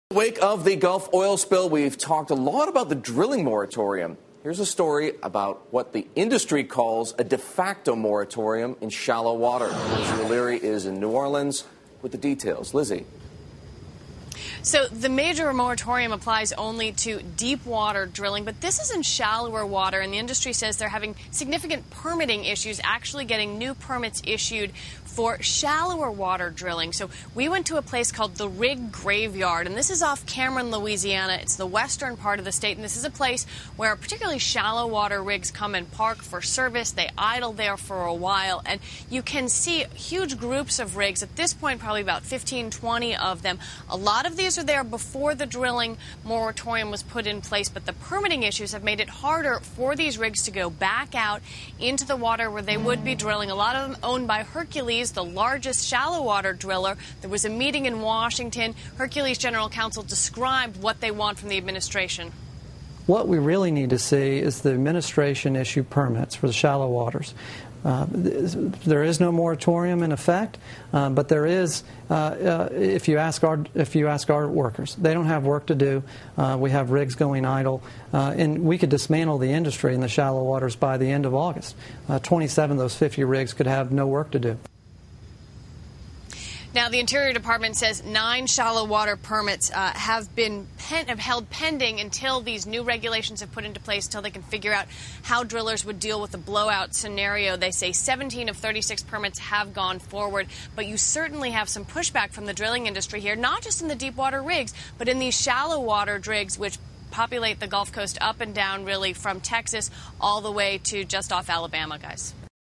Shallow Water Drilling: Bloomberg News on De Facto Moratorium
Bloomberg News reports from a "rig graveyard" in Cameron, Louisiana where just offshore shallow water drilling rigs sit idle as the Department of Interior holds up approval of permits for new wells. In this video, Jim Noe of the Shallow Water Energy Security Coalition says that without permits, the de facto moratorium could "dismantle" the shallow water industry.